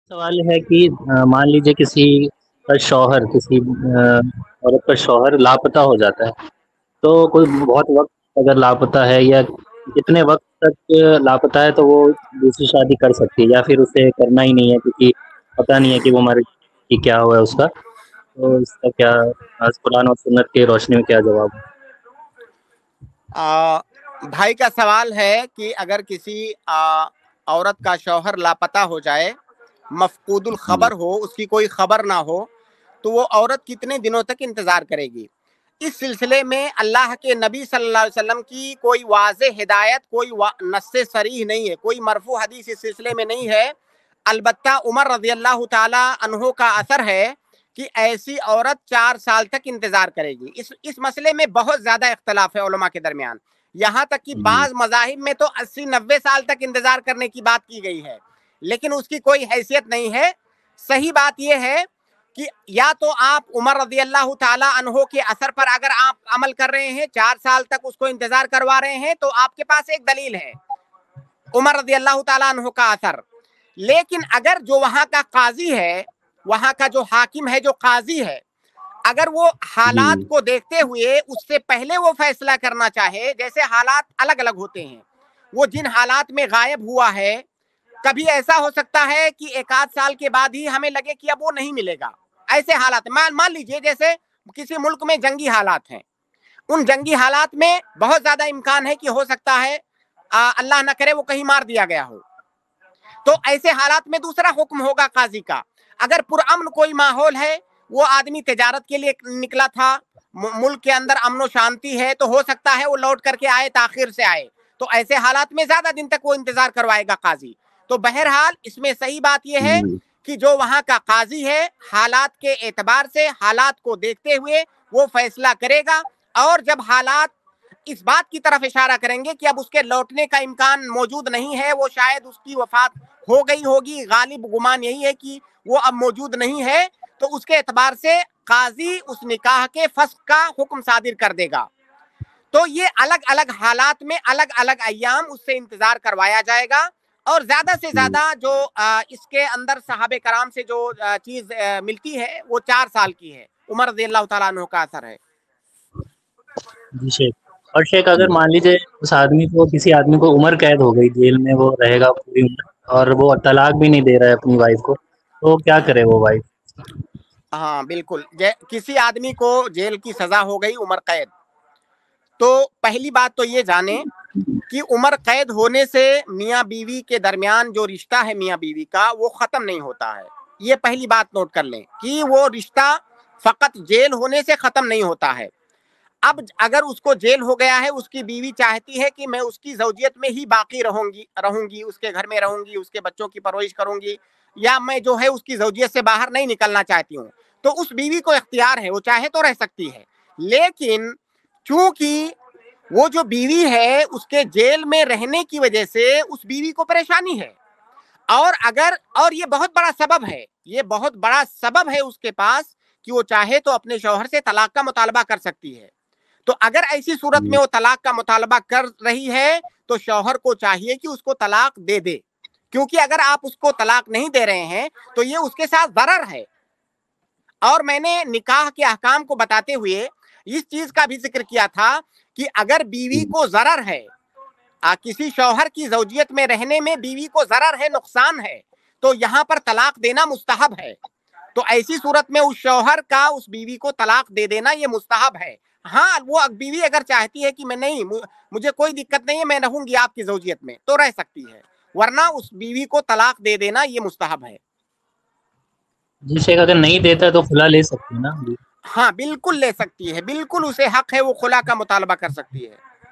Agar Kisi Ka Shohar Bahut Waqt Tak Lapata Ho Jaaye Toh Aurat Kab Tak Intezar Kare? By Dr. Naseem
Agar Kisi Ka Shohar Bahut Waqt Tak Lapata Ho Jaaye Toh Aurat Kab Tak Intezar Kare? By Dr. Mohammad Naseem Madani iPlus TV Deeni Q&A

UPI : donateilm@icici